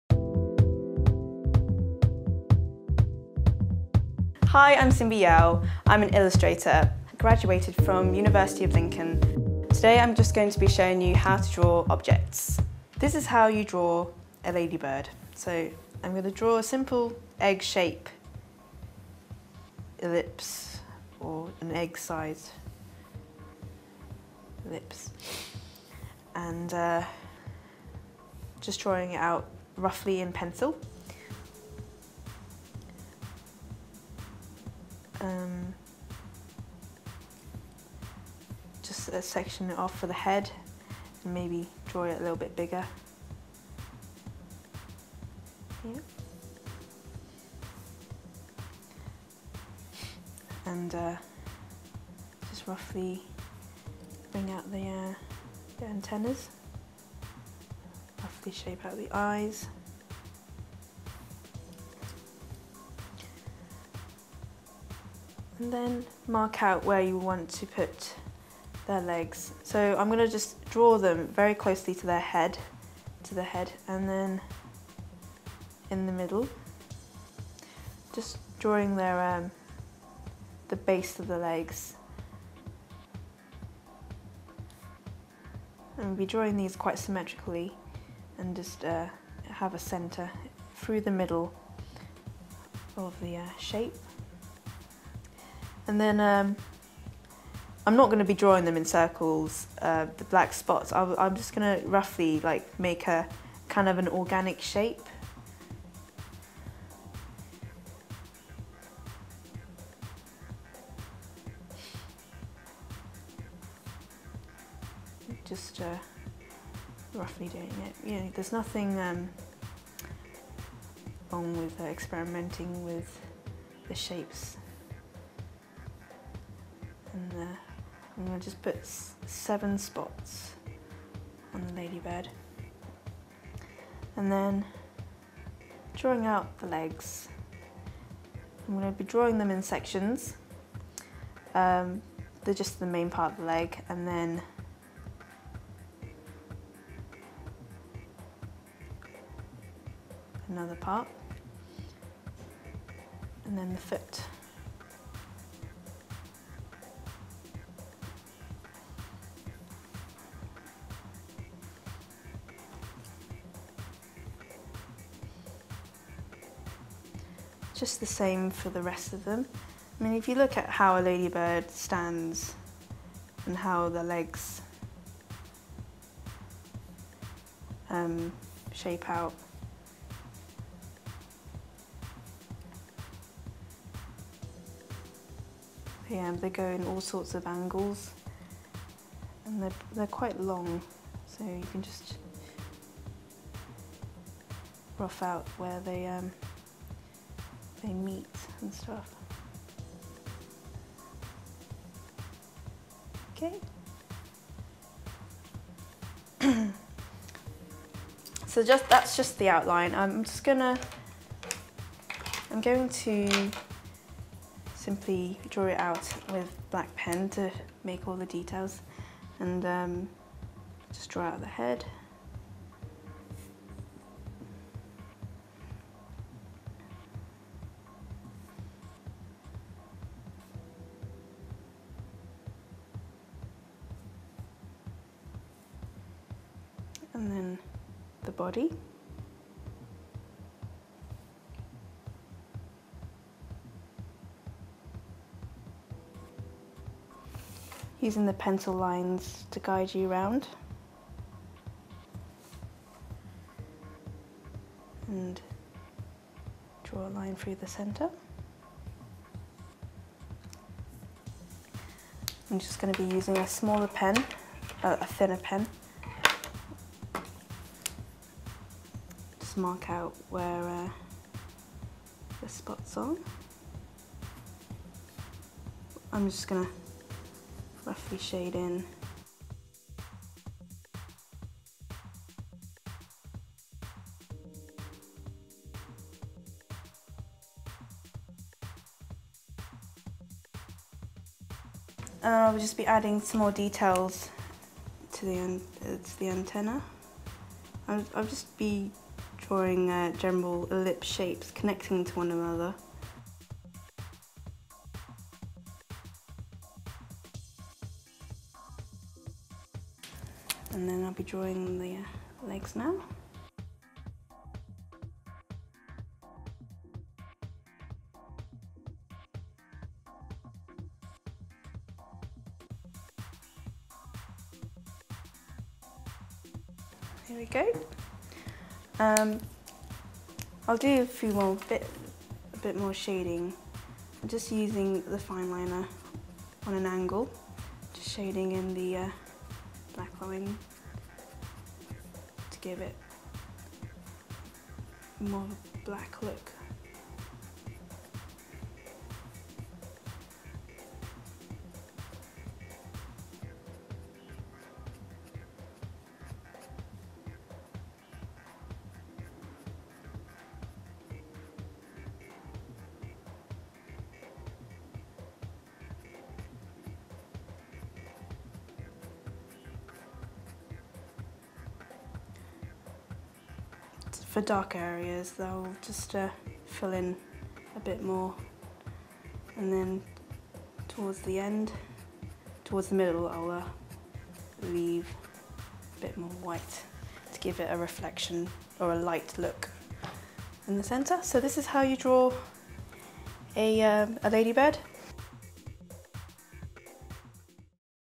How To Draw A Ladybird
A video lesson on How To Draw A Ladybird that will improve your drawing and sketching skills. Learn how to get good at drawing and sketching from Videojug's hand-picked professional experts.